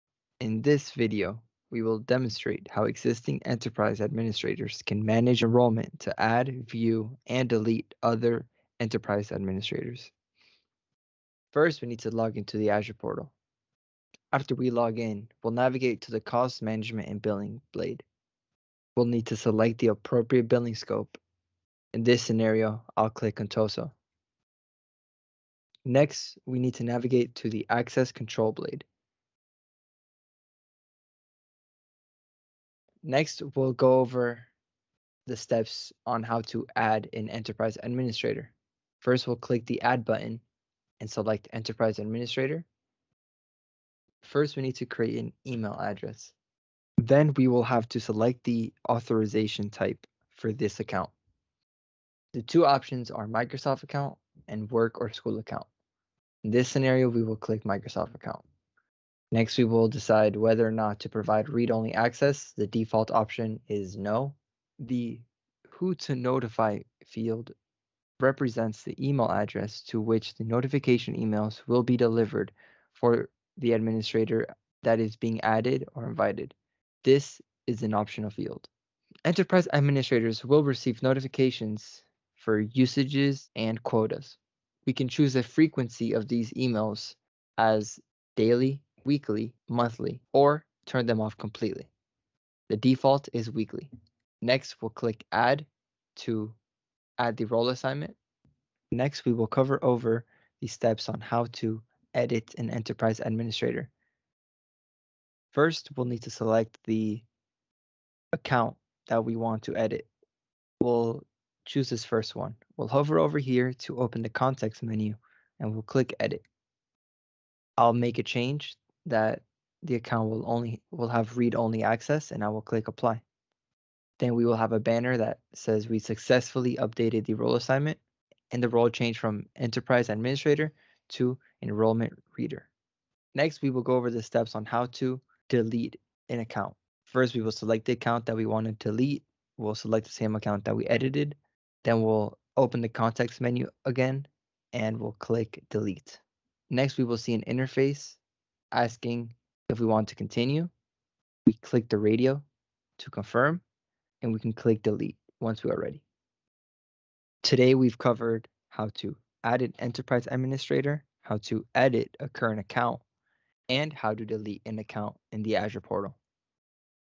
Managing Enrollments in the Azure Portal
Enterprise Azure customers can now manage their billing directly from the Azure Portal.
In this video, you will learn how an existing Enterprise Administrator can manage their enrollment or billing account in the Azure portal.